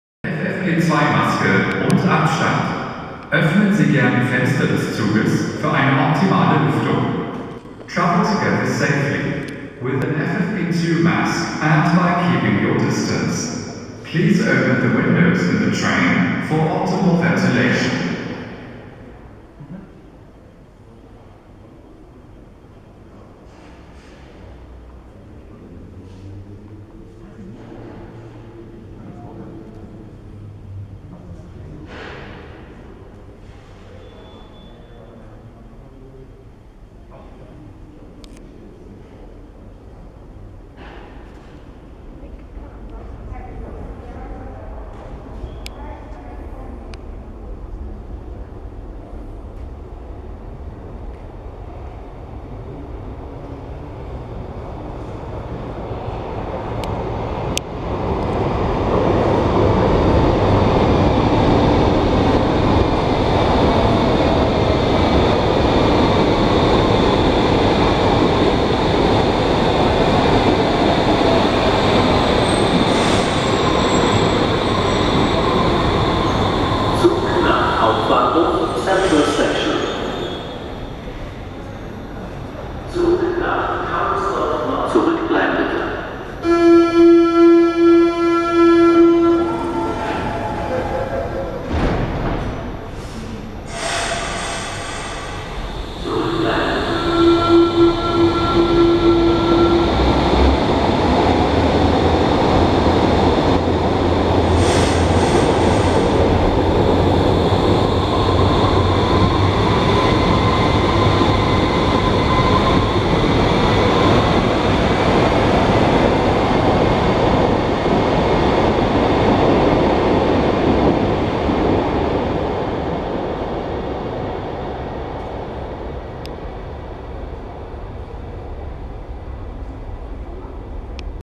U-Bahnhof Rotes Rathaus, Berlin
Neue Berliner U-Bahnstation Rotes Rathaus (Eröffnung Dezember 2020) im Ortsteil Mitte. Diese Station ist Teil der Verlängerung der Linie U5 zwischen Alexanderplatz und Brandenburger Tor. Somit ist die U5 nun komplett vom Hauptbahnhof bis Hönow durchgehend befahrbar. Aufgenommen am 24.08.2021.